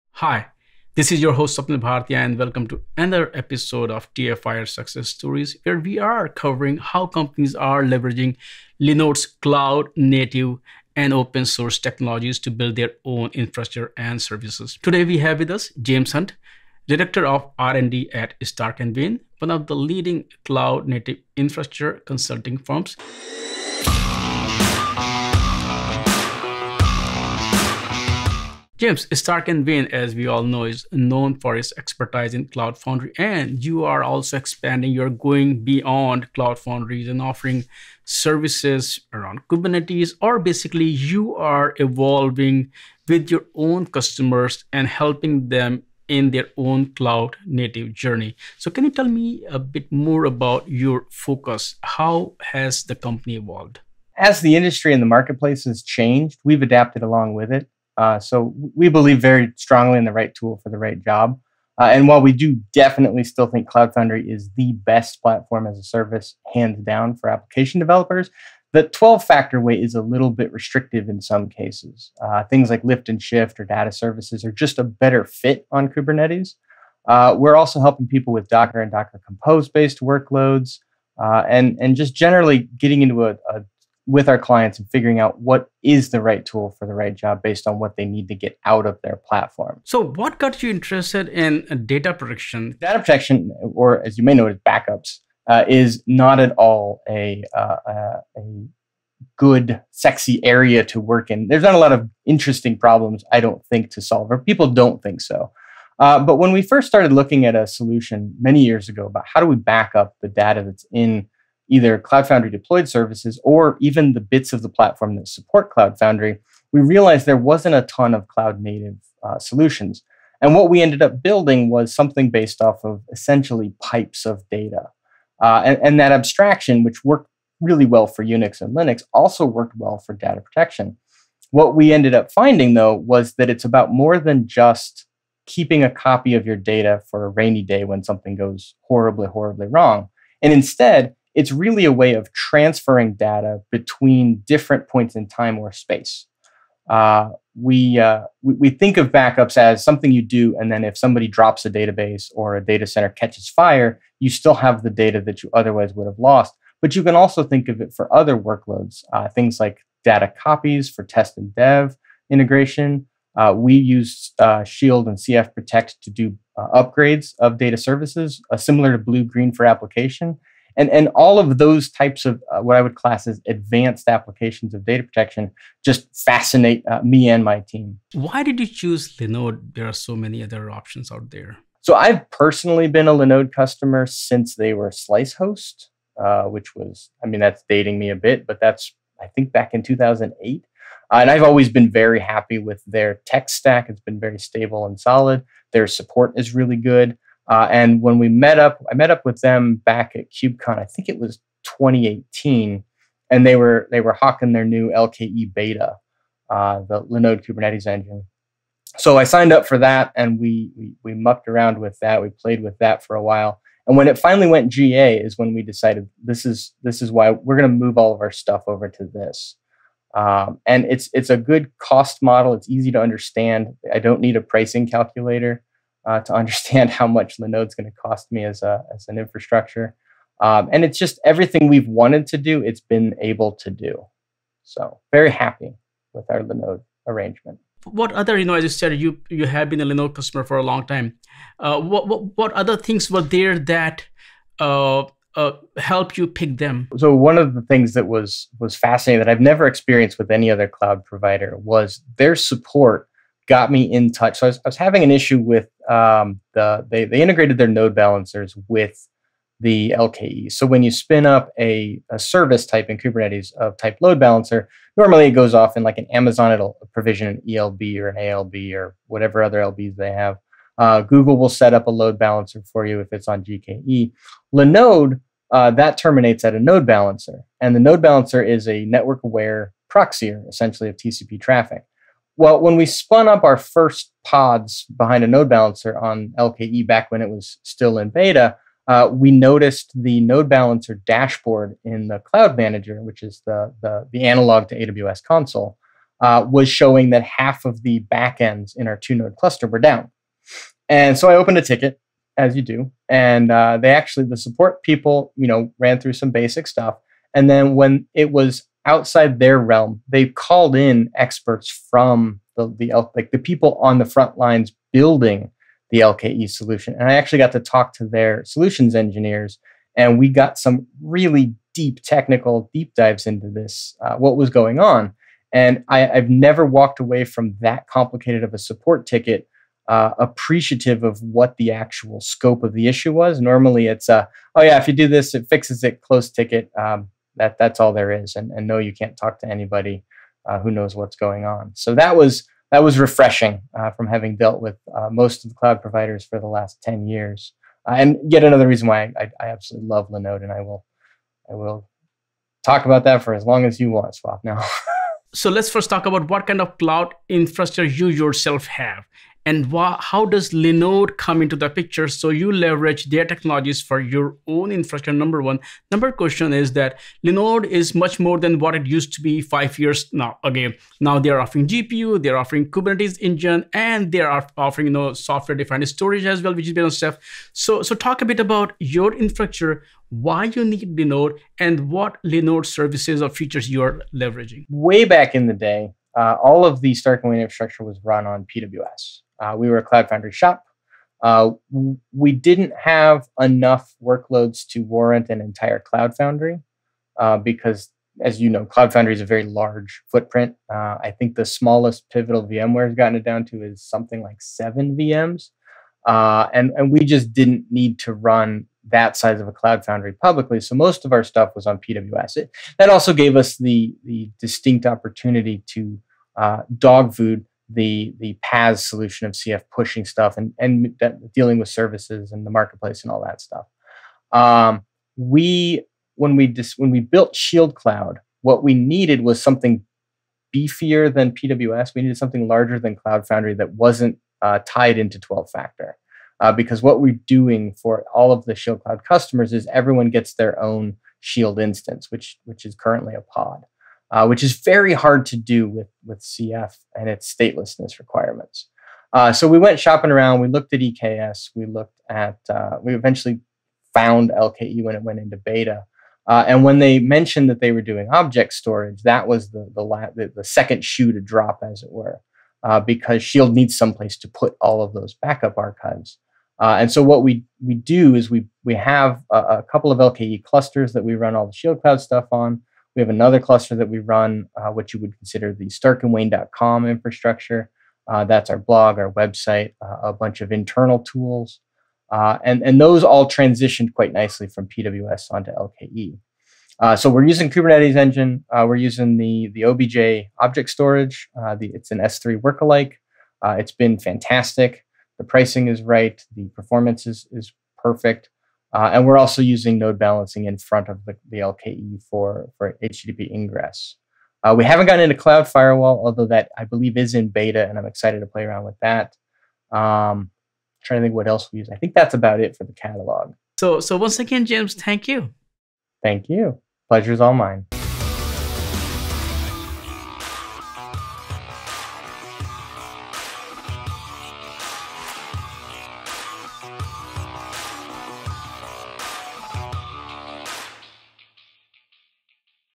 Linode Is Able To Do Everything We Want To Do: James Hunt, Stark & Wayne
In this episode of TFiR Success Stories, James Hunt, Director of R&D at Stark & Wayne joins us to talk about why they chose Linode over other players, including hyperscalers like AWS.

“I've always been very happy with their tech stack. It has been very stable and solid. Their support is really good,” said Hunt. He also appreciates Linode’s cost model. “It's easy to understand. I don't need a pricing calculator to understand how much Linode is going to cost me as an infrastructure. Everything we have wanted to do; it's been able to do.”